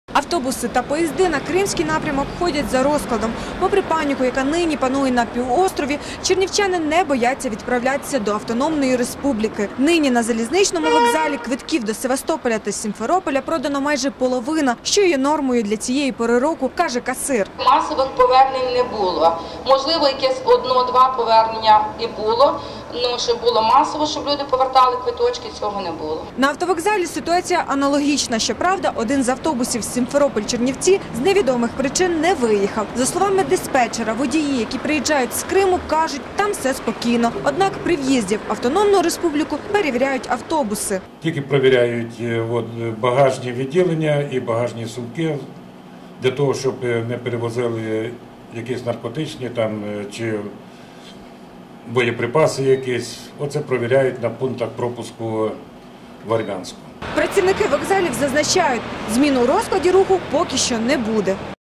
140304 До Криму без перешкод
З Чернівців до Криму, наразі, можна дістатися  без перешкод.